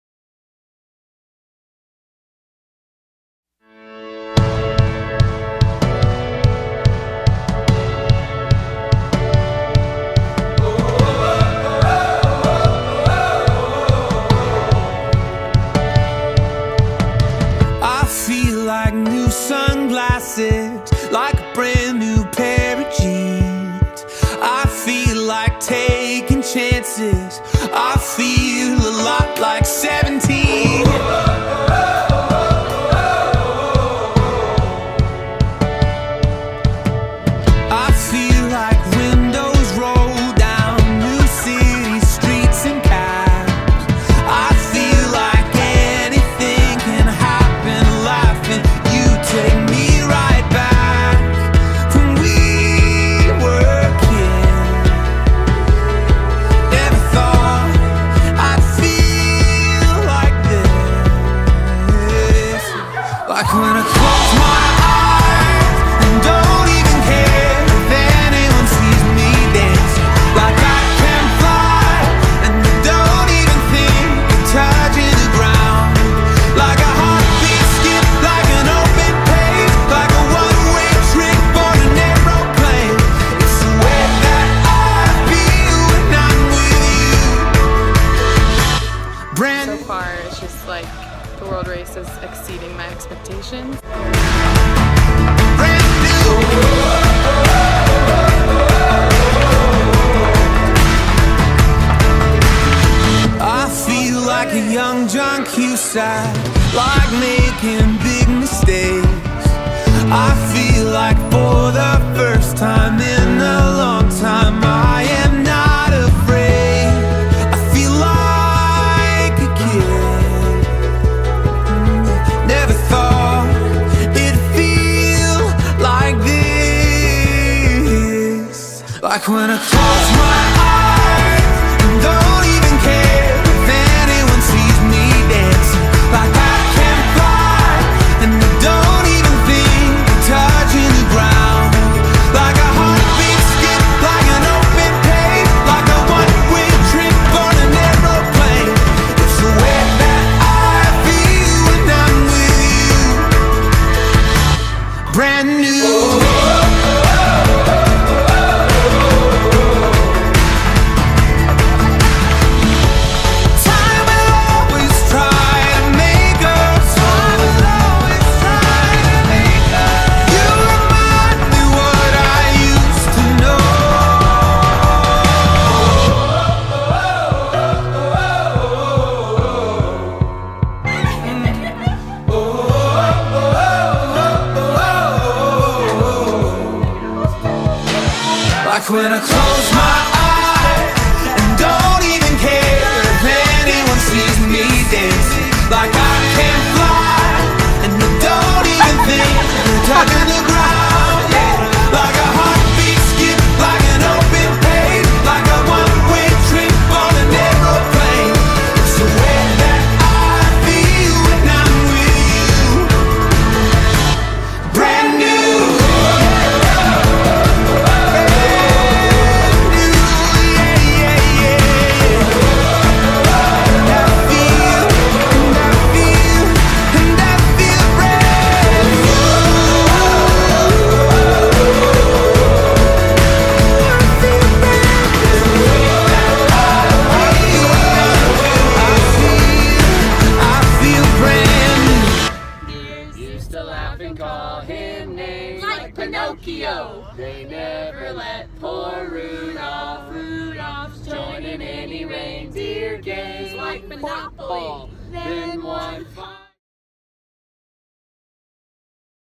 Worldrace - South America
Highlights from my first 3 months on the worldrace!